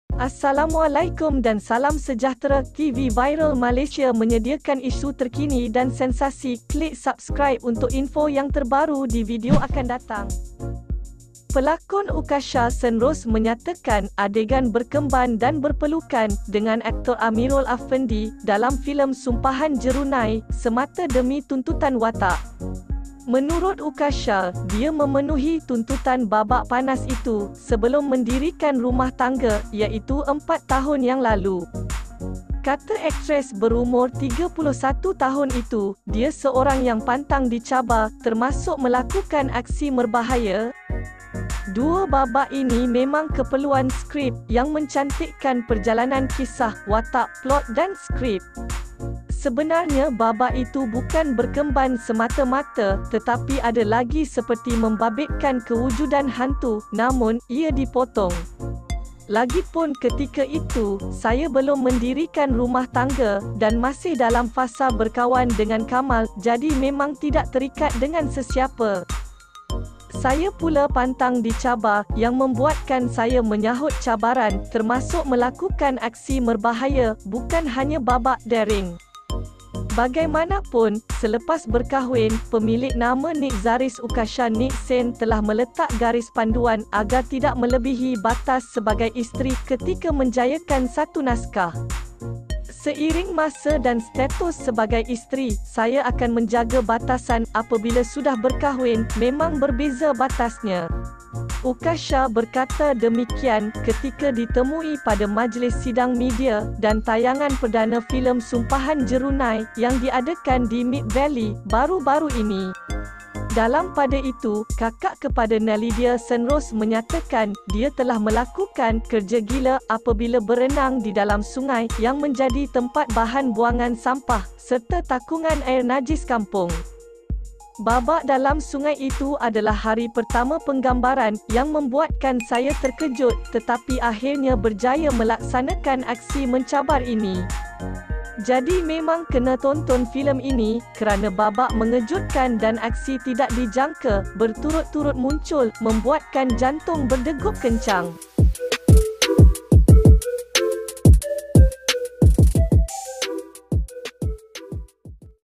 UQASHA SENROSE JAWAB ISU ADEGAN BERKEMBAN & PELUK AMERUL AFFENDI...
Pelakon Uqasha Senrose menyatakan, adegan berkemban dan berpelukan, dengan aktor Amerul Affendi, dalam filem Sumpahan Jerunei, semata demi tuntutan watak.

Menurut Uqasha, dia memenuhi tuntutan babak panas itu, sebelum mendirikan rumah tangga, iaitu empat tahun yang lalu.